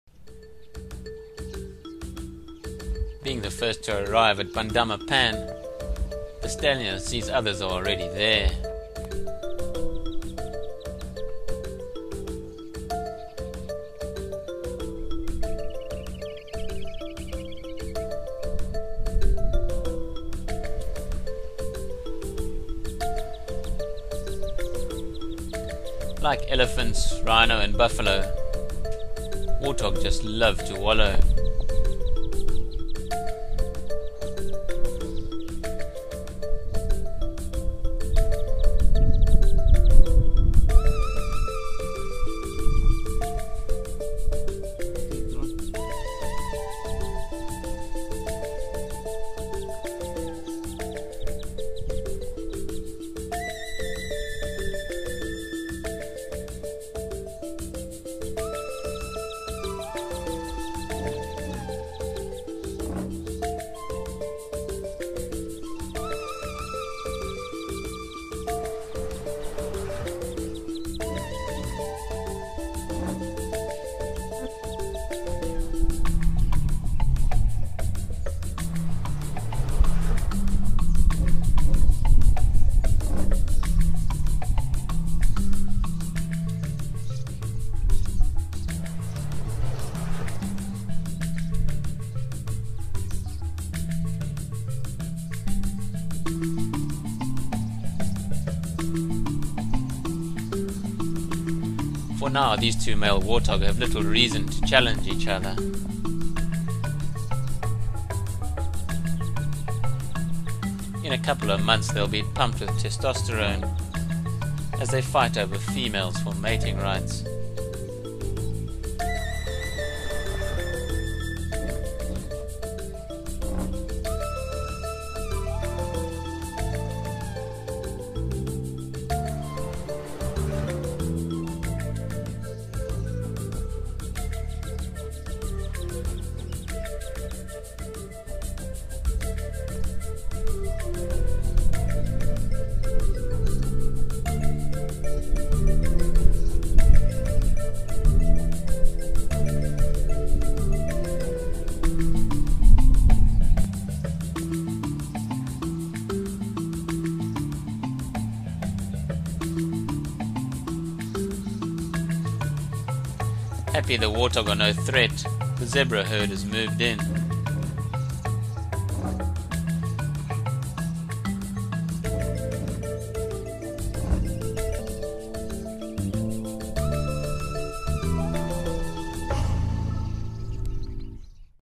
Warthog Wallow
Arriving first at Bandama the stallion is nervous to approach. 2 male warthogs are already busying themselves in the mud wallow. As they move out they confront each other in an aggressive but friendly way for now. Seeing all is fine at the water the zebra move in to drink.